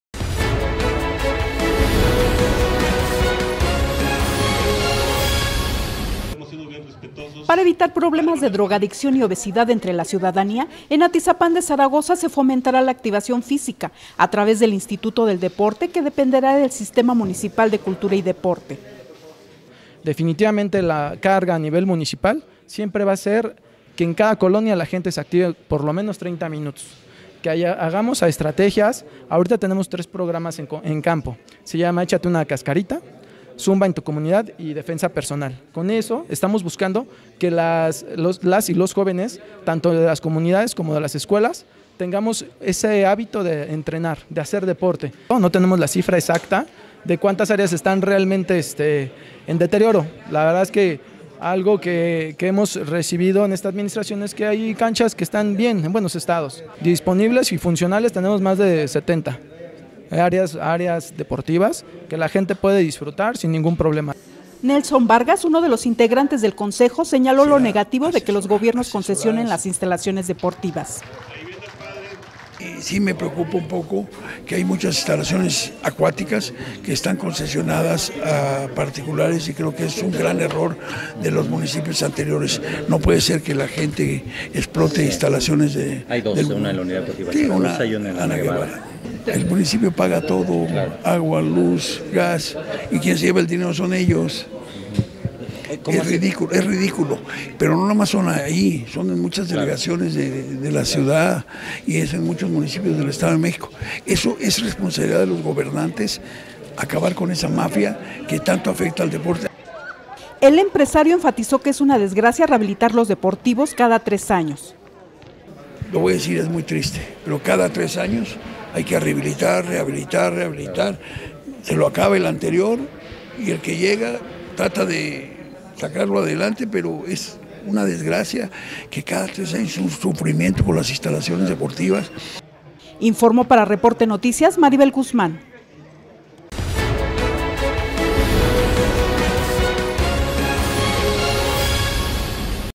Gobiernos deben erradicar la concesión de deportivos:Nelson Vargas
Para evitar problemas de drogadicción y obesidad entre la ciudadanía, en Atizapán de Zaragoza se fomentará la activación física, a través del Instituto del Deporte que dependerá de la dirección de Cultura y Deporte, a cargo Edgar Israel Fortanel Soto.